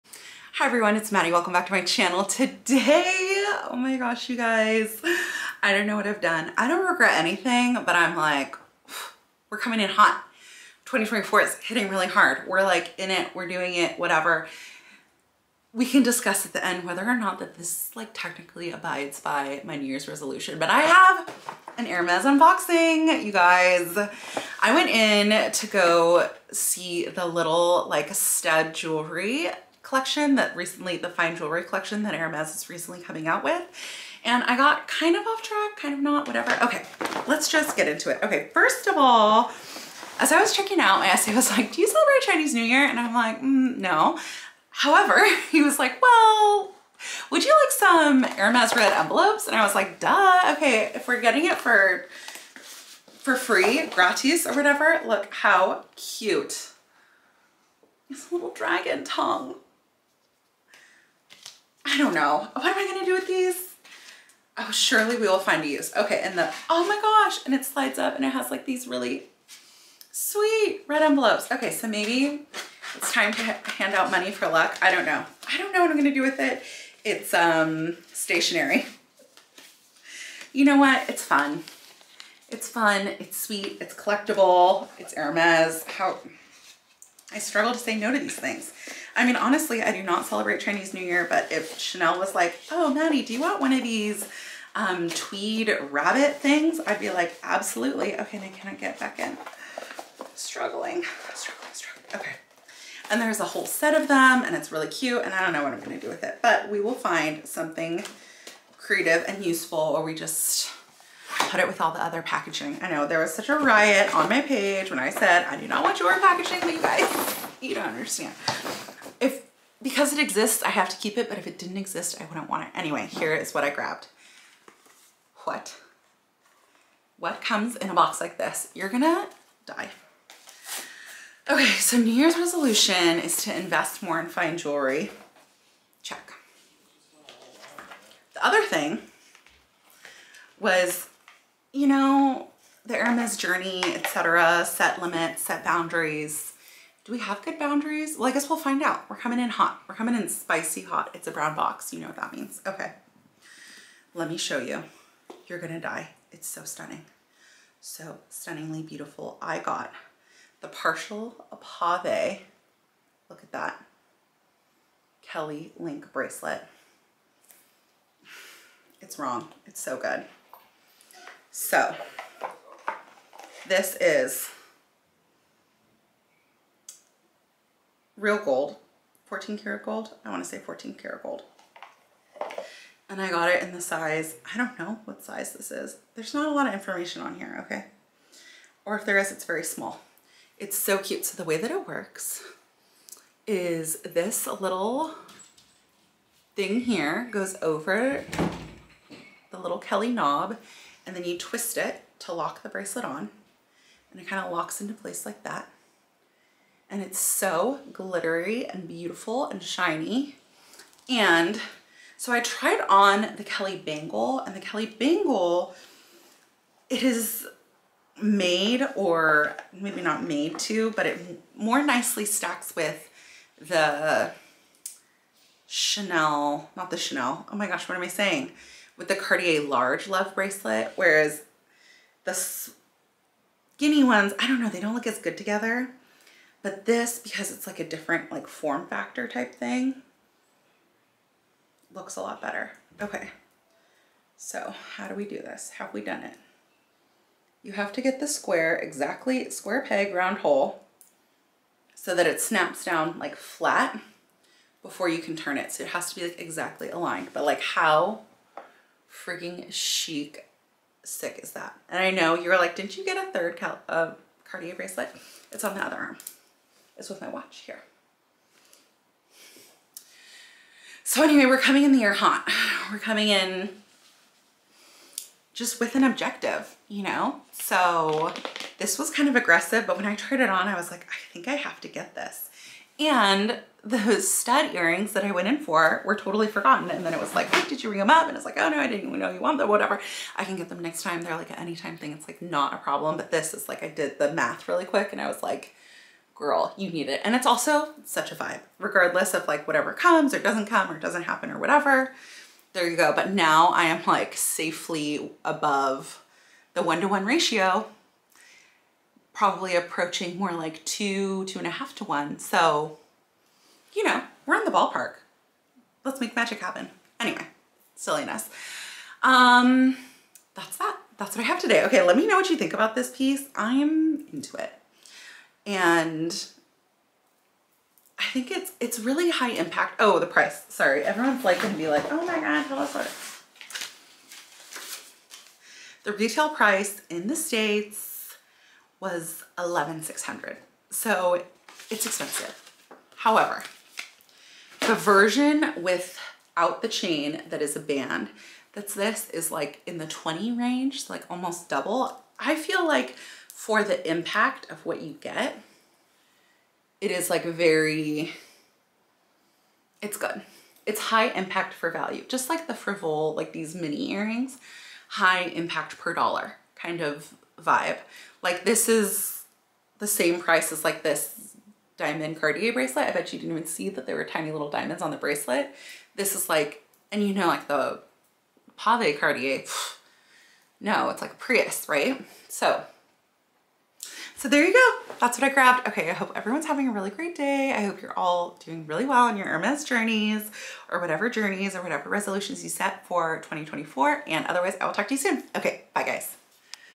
Hermes first purchase of the year... and it's a big one! Working on my Hermes Journey!
💗💗 Open me!

Welcome, my name is Maddie and I make videos about shopping, luxury, and fashion and everything in between.

I hope you enjoy this video on my first purchase for 2024! I'm continuing on my H journey!